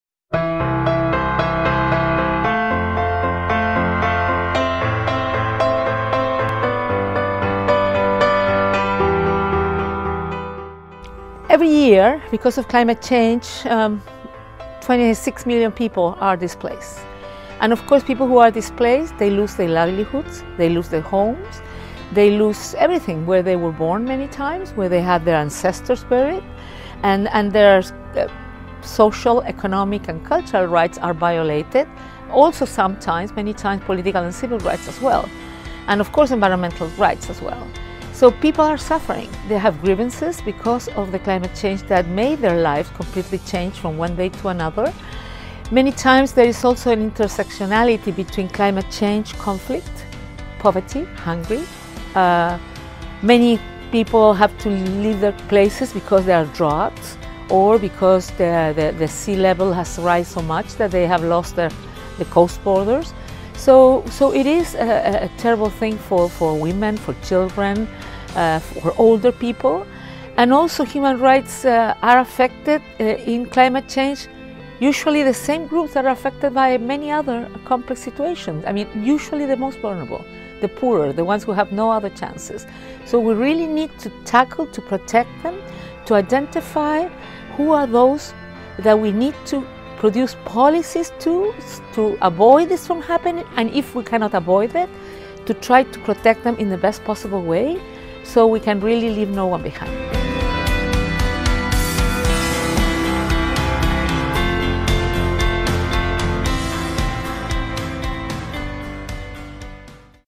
Michelle Bachelet raises alarm on the impact of climate change on Human Rights
United Nations High Commissioner for Human Rights Michelle Bachelet answers to the question : how are Human Rights affected by Climate Change ?